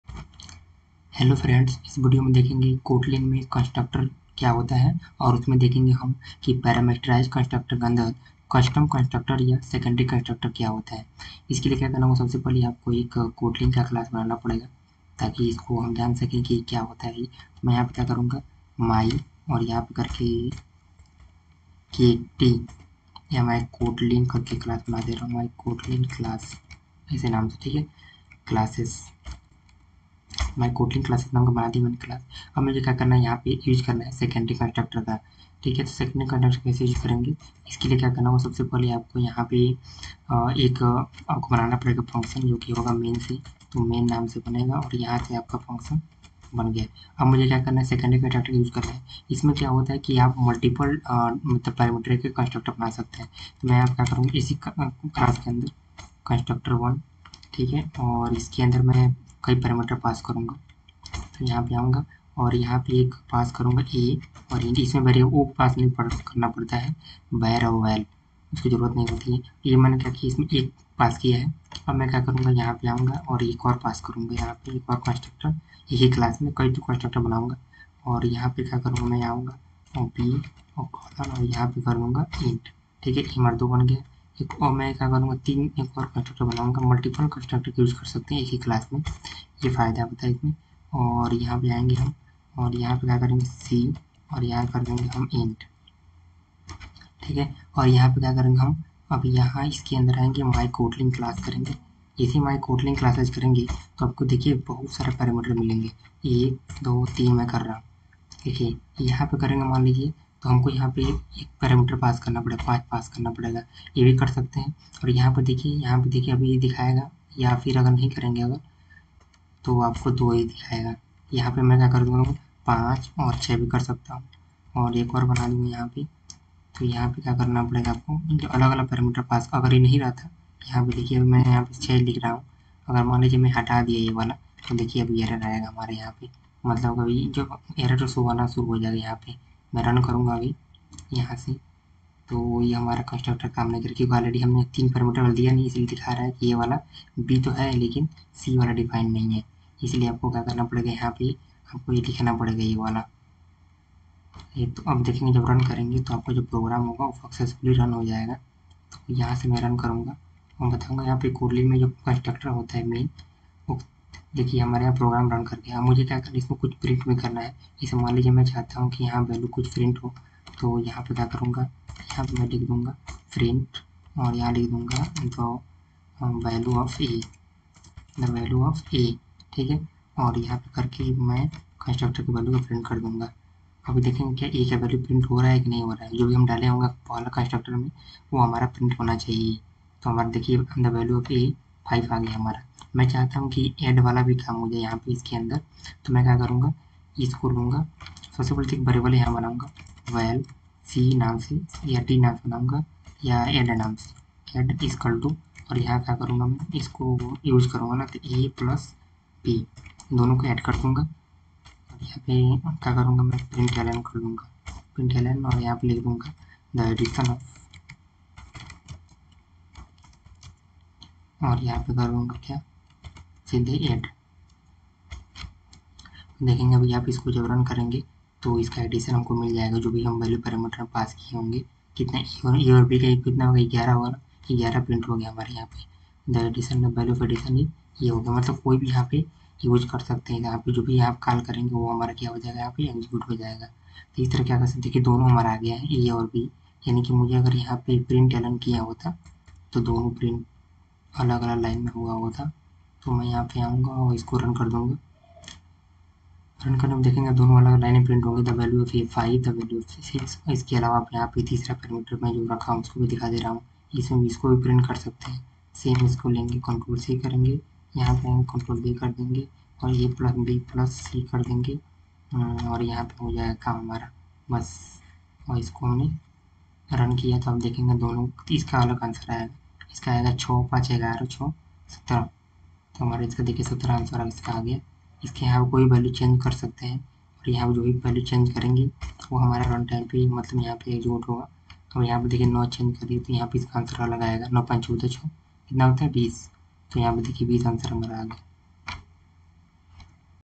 How to use Custom or Secondary constructor in Kotlin Android Studio
In this video, I want to show How to use Custom or Secondary constructor in Kotlin Android Studio